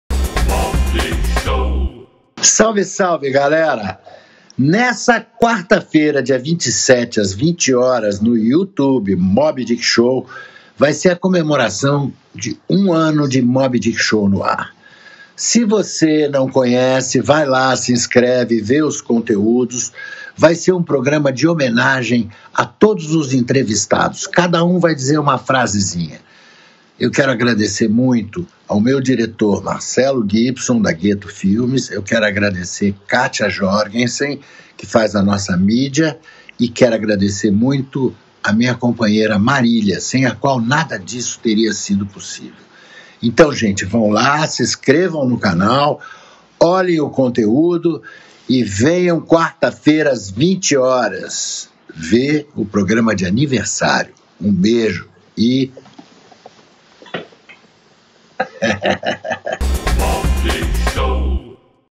Mobydickshow especial de aniversário (1 ANO) - TRAILER Mobydickshow 53
TRAILER da próxima entrevista do Mobydickshow que vai ao ar no dia 27 out 2021, às 20h.